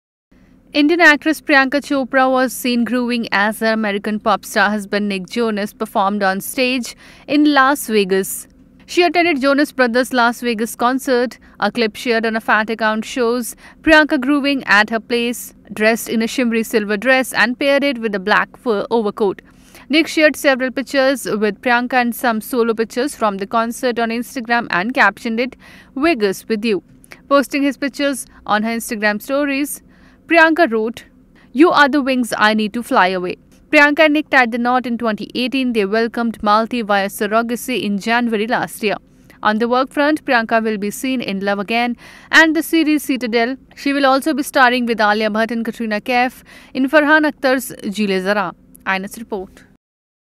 Priyanka Chopra dances as hubby Nick Jonas performs on stage in Las Vegas
Indian actress Priyanka Chopra was seen grooving as her American pop star husband Nick Jonas performed on stage in Las Vegas.